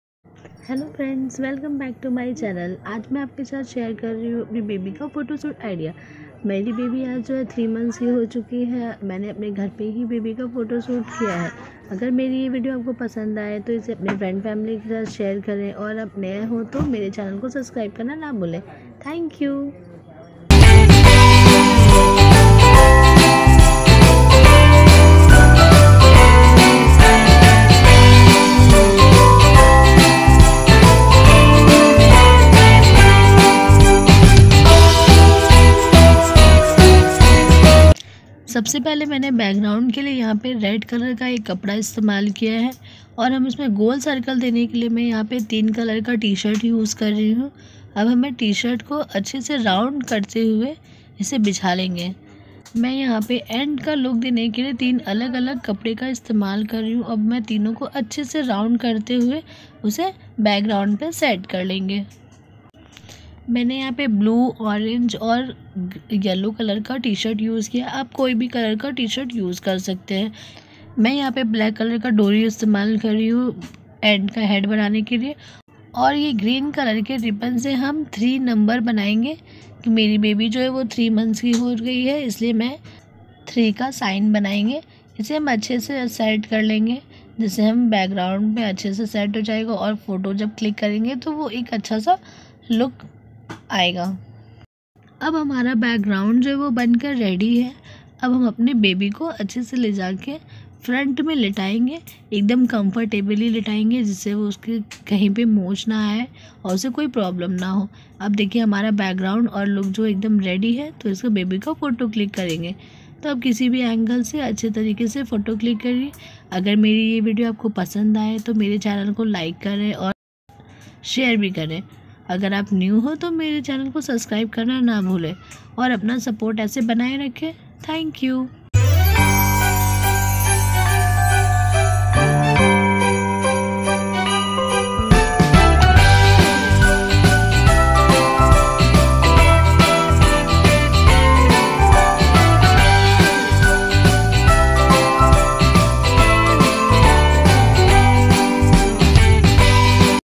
Baby photoshoot ideas at home || 3 month baby photoshoot at home | Monthly Photoshoot at Home
Hello friends
aaj main iss video aapko apne baby ka Photoshoot Ideas share kr rhi hu ki maine kaise kiya .
iske liye aapko bhut sari cheez ki jrurat nhi hogi jise aapke ghar me aasani se mil jaayegi.
agar aapko meri video pasand aaye to ak like jrur kre aur meri video ko aapne frnd, family ke sath share kare.
apna pyar aise hi bnaaye rakhe .